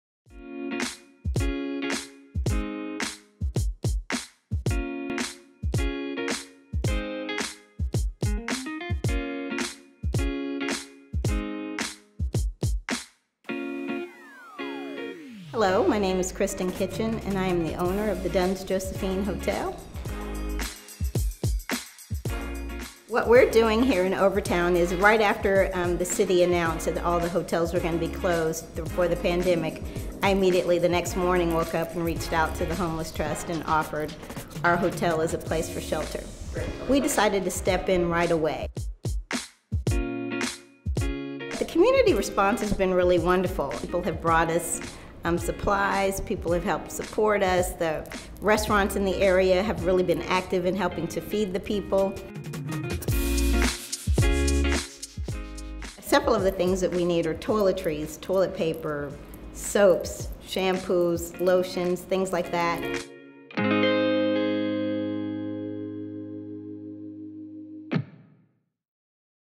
Miamians Who Shine | Kristin Kitchen
The Greater Miami Convention & Visitors Bureau is showcasing amazing examples of individuals who have made Miami Shine during the COVID-19 pandemic. Today's feature is Kristin Kitchen, owner of Dunns-Jospehine Hotel in Historic Overtown.